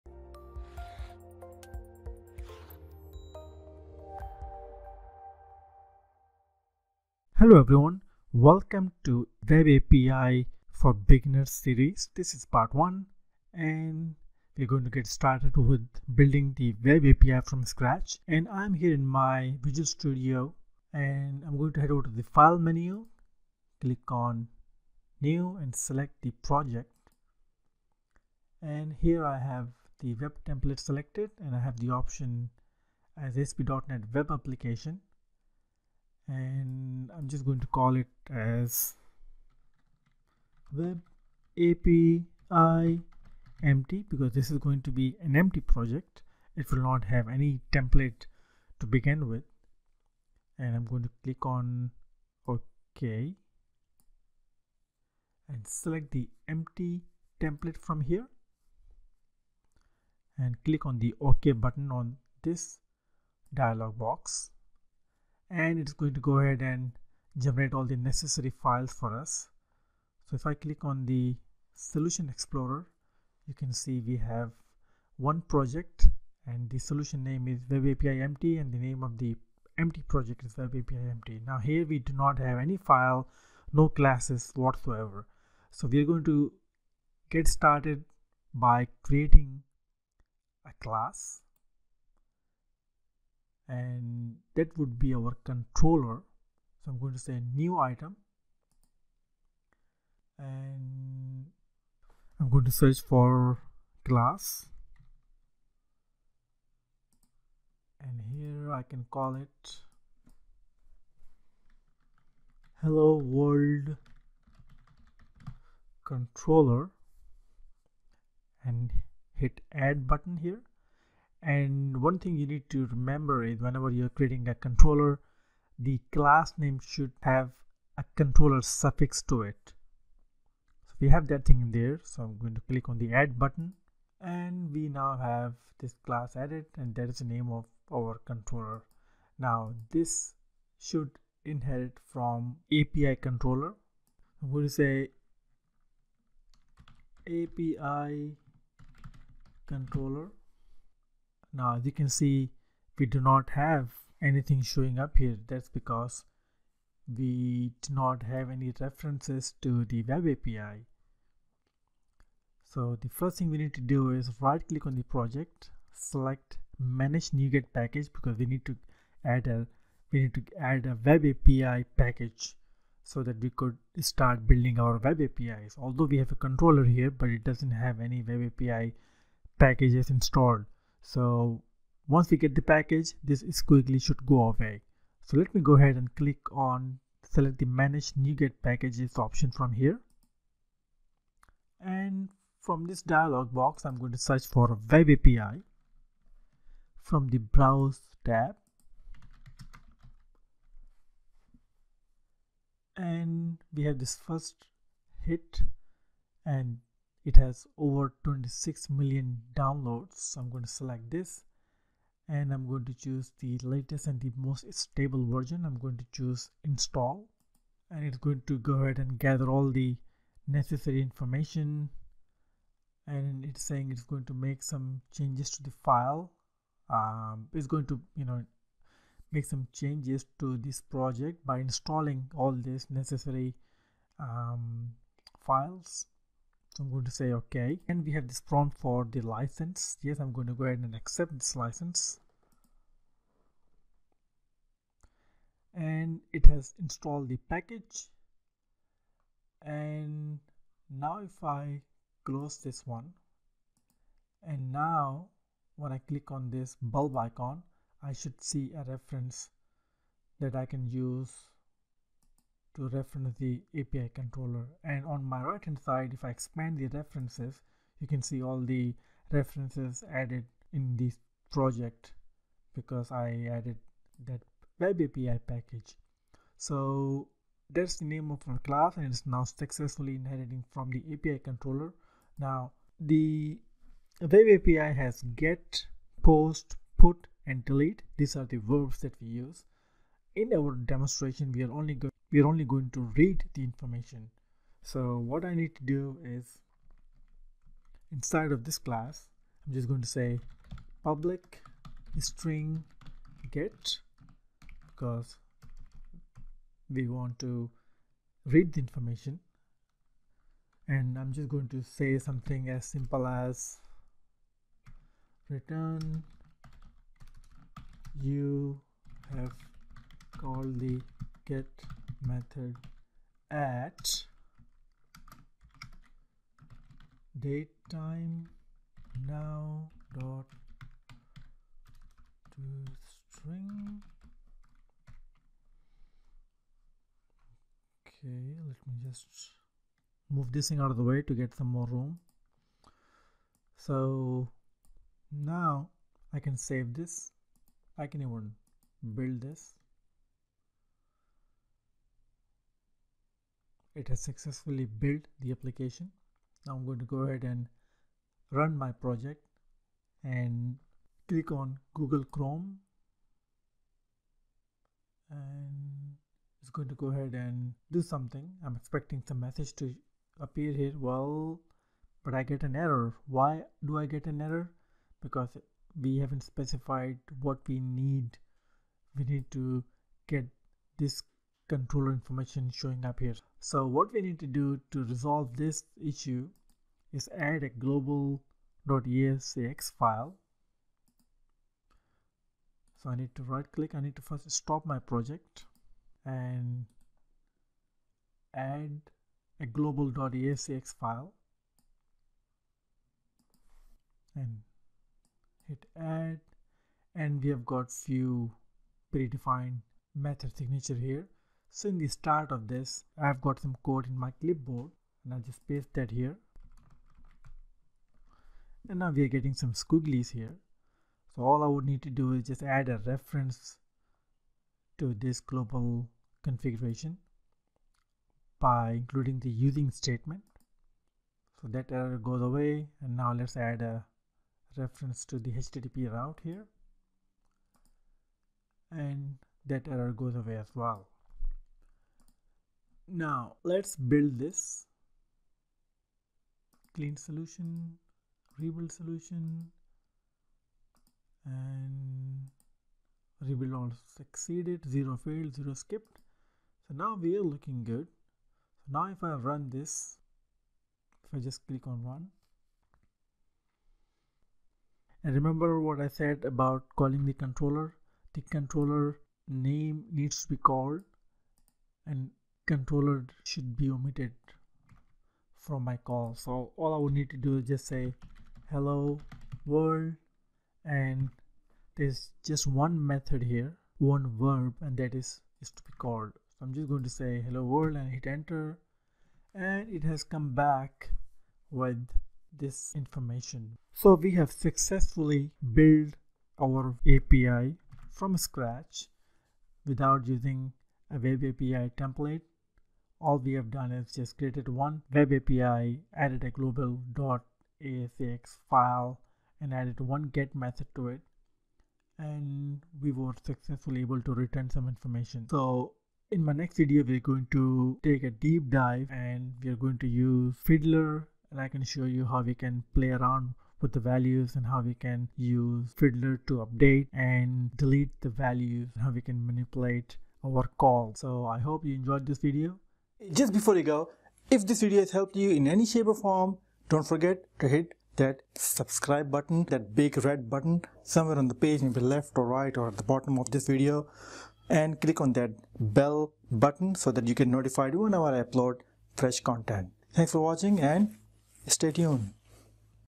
How to Create Web API from scratch - Part 1
Check out the full course on Web API in ASP.Net Core
Web API in ASP.Net Core: A Project-Based Step by Step Course

In this video, I explain to you how to create Web API from scratch using an empty project.